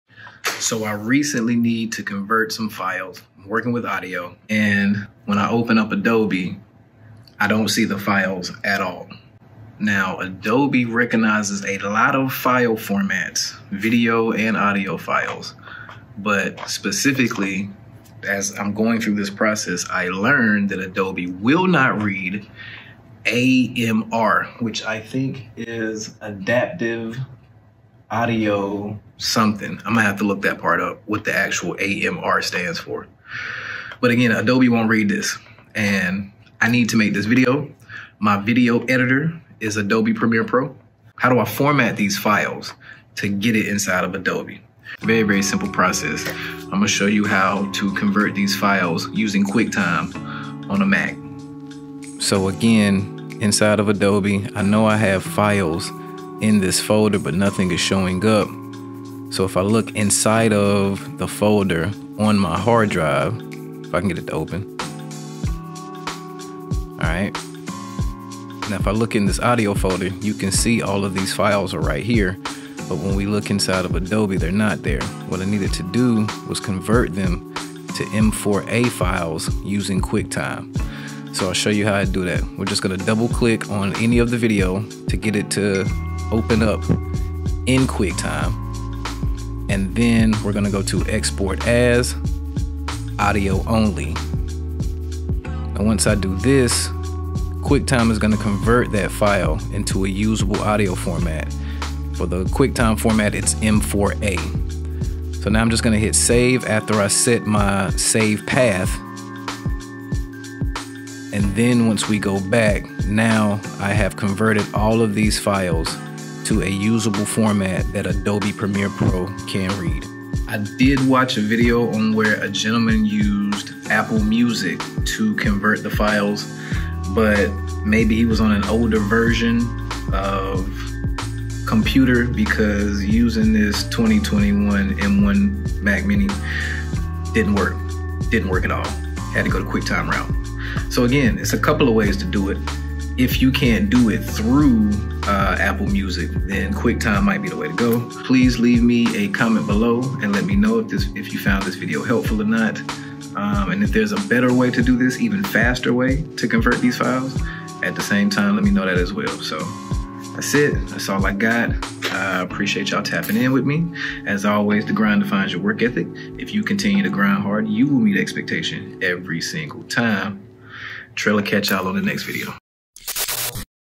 How to convert AMR audio files on Mac
I searched YouTube for this information and couldn't find one video about how to achieve this on an Apple computer, so This may be a first! I needed to use some audio taht was saved from phone calls, and it came as amr format.

Adaptive Multi Rate (AMR) is an audio compression file type designed by Ericsson in the 1990's. Originally created for use on 3G cell phones, amr is designed to optimize human speech data.

My video editing software is Adobe, and when I opened the folder to import the audio, it wasn't recognized. I went back to the folder on my hard drive and tried to preview a file, when I learned QuickTime would actually read the format, and I discovered a way to convert the audio files using QuickTime.

So, if you ever need to convert some old audio type, don't overlook QuickTime,

and if you're on a windows machine, you can do the same thing with your VLC player.